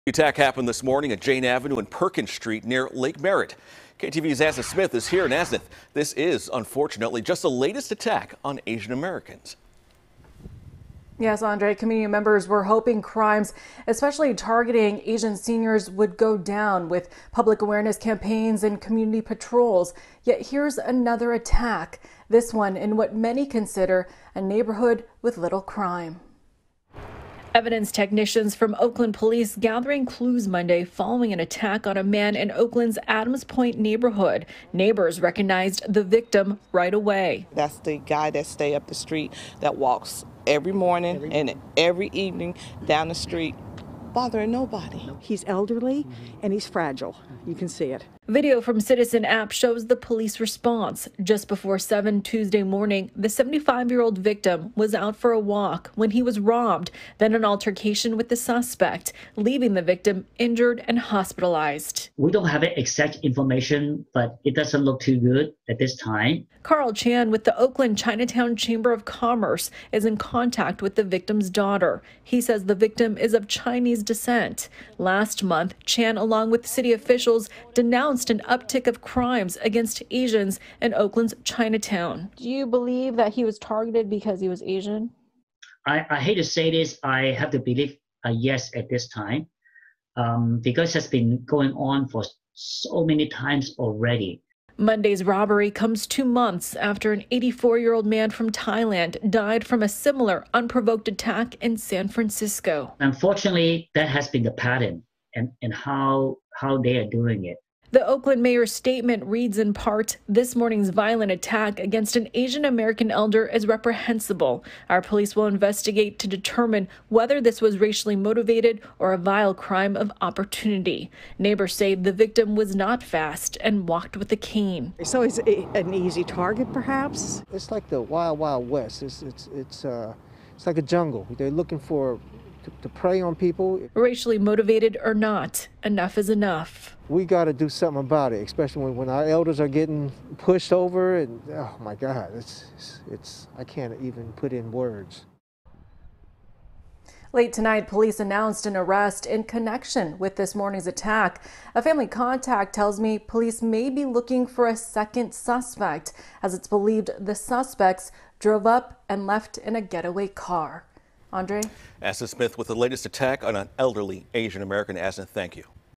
Arrest made in strong-arm robbery of elderly man in Oakland
Oakland police said on Tuesday they made an arrest in a strong-arm robbery of an elderly man in the Adams Point neighborhood. It's the latest in a string of attacks targeting the Asian-American community. Family members say the victim's condition is not looking good.